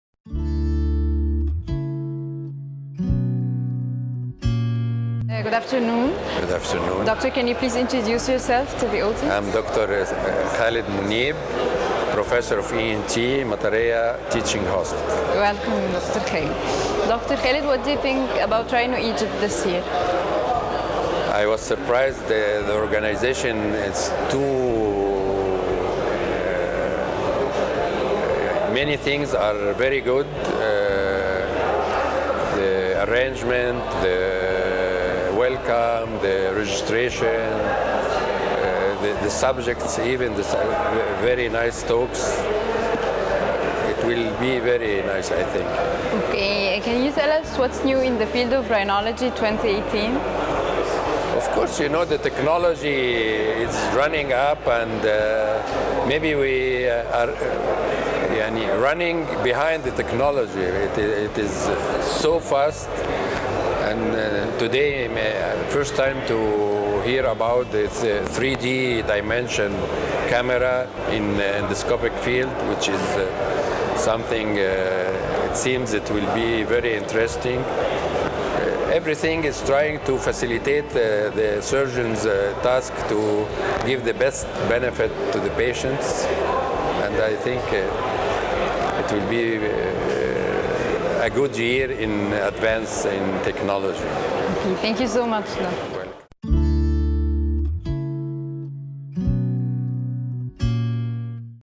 Dr.Khaled Monib Talks about RhinoEgypt2018.
Professor of full-time ENT at Al-Matareya Teaching Hospital.
استاذ متفرغ انف واذن وحنجرة بمستشفي المطرية التعليمي.